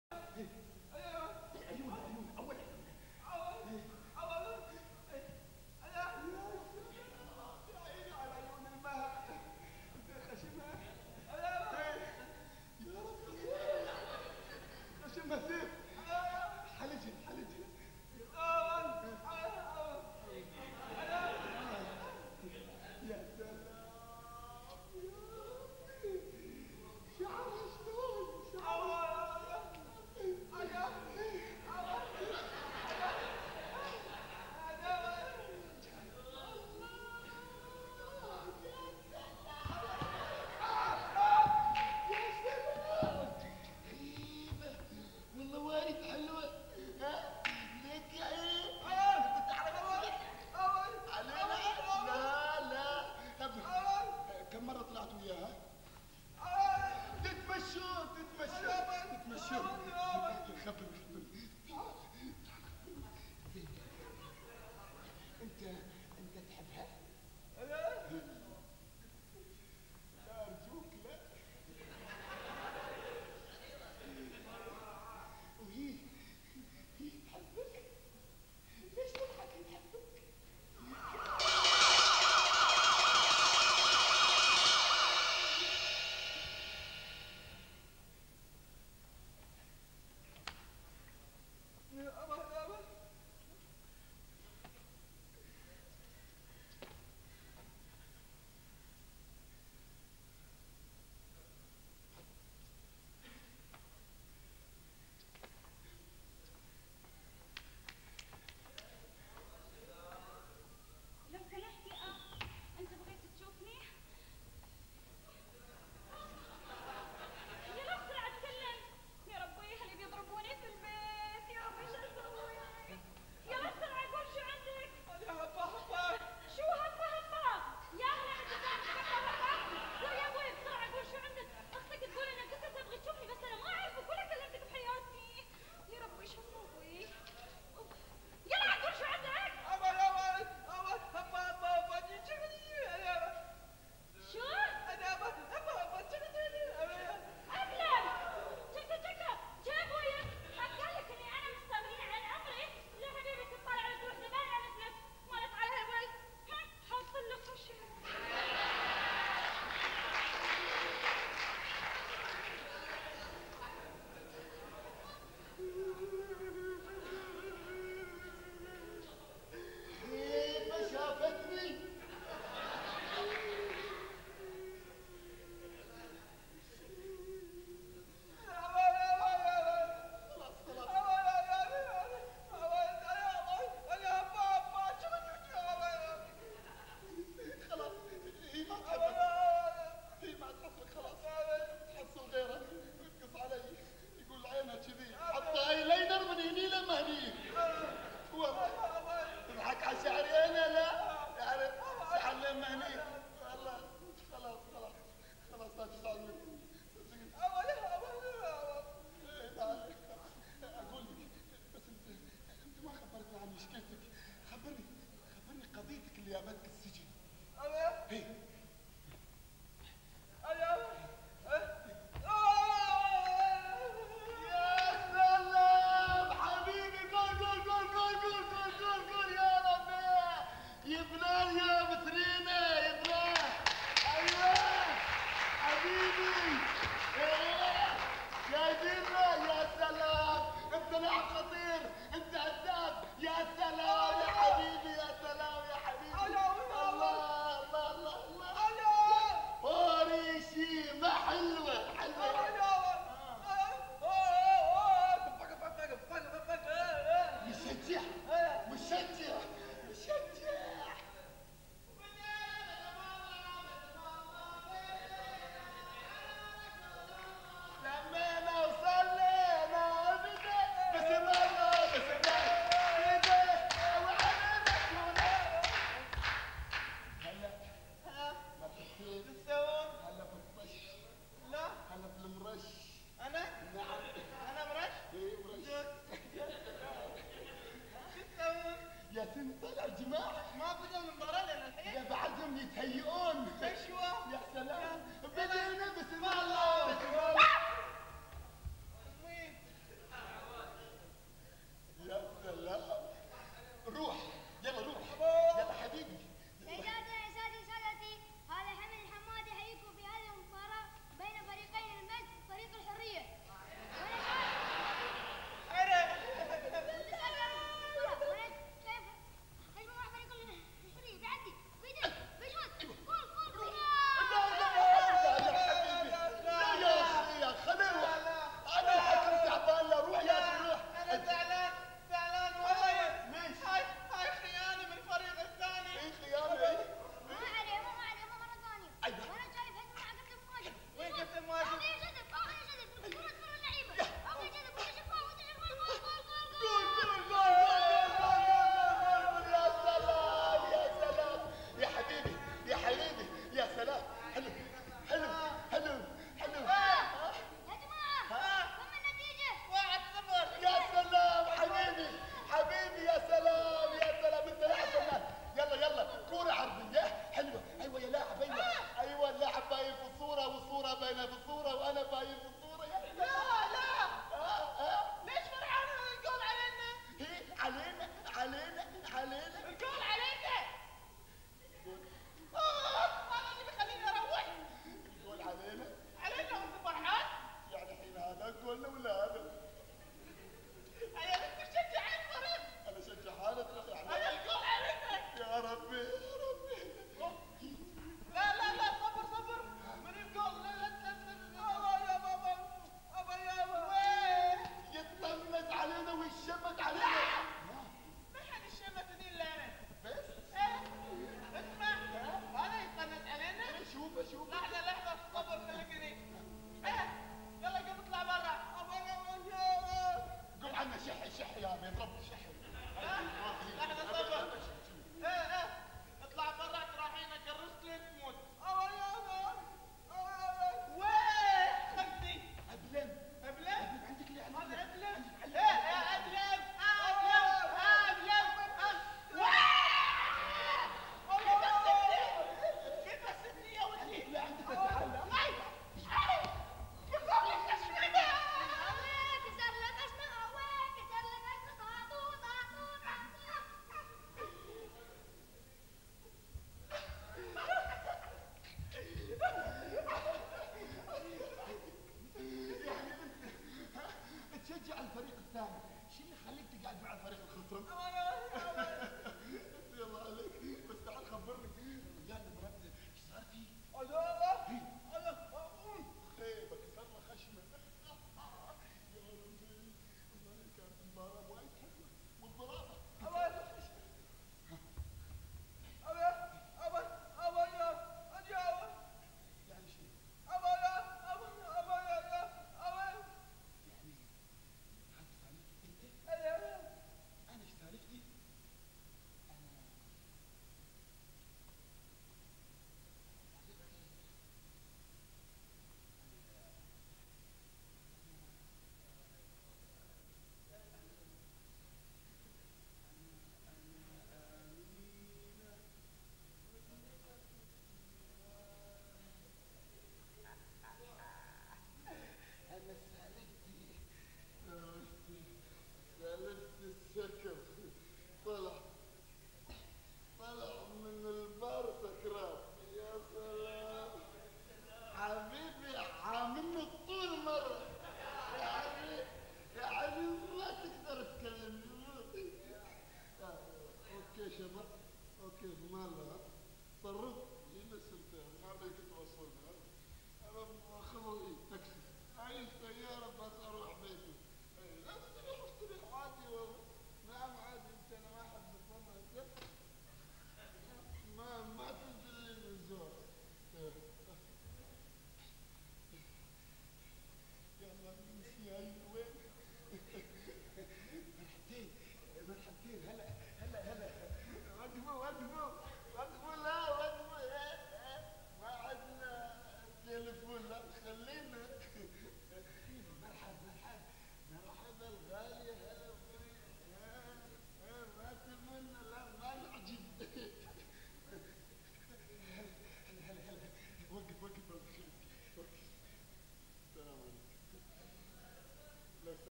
Part5 ١٩٨٩ البقعة
مسرحية البقعة ١٩٨٩
إخراج: أحمد الأنصاري

"The Spot"
Directed By: Ahmed Al Ansari
Youth Theatre & Arts Production 1989